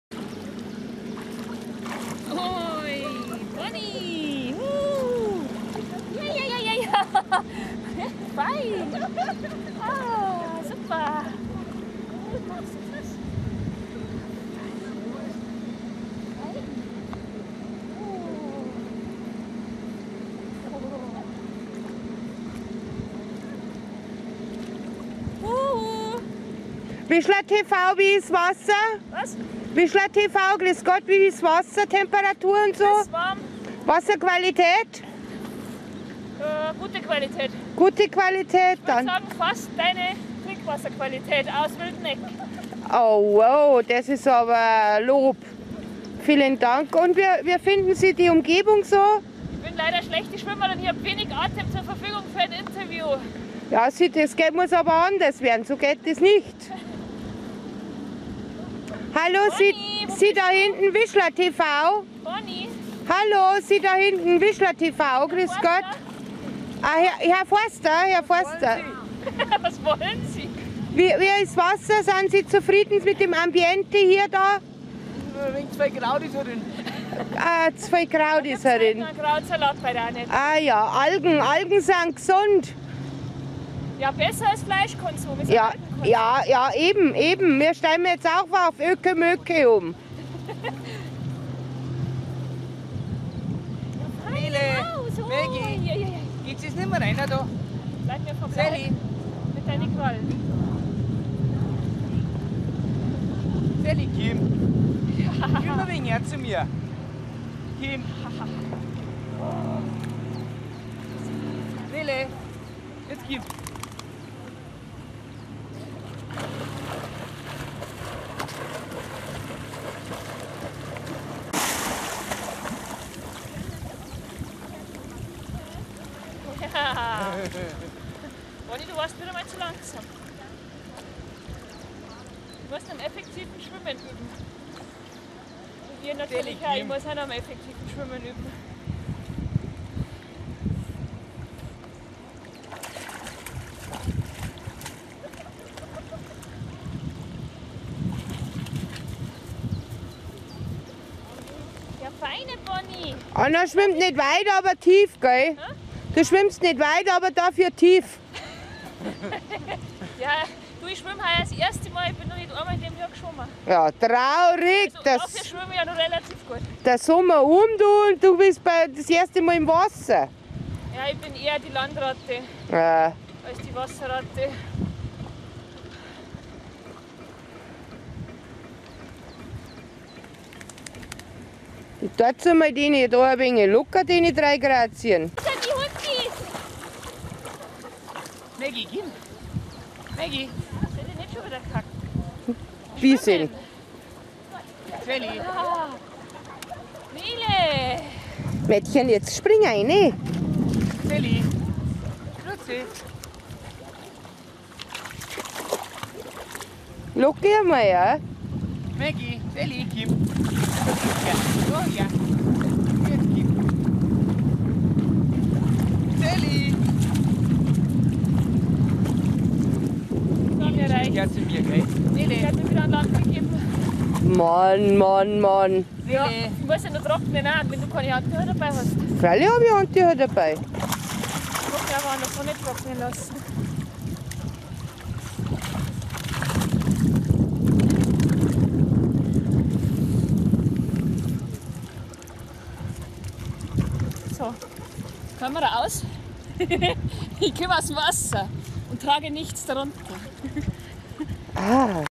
vizsla-tv.mp4
vizsla-TV beim Interview von Badegästen am Ettlinger Weiher ;)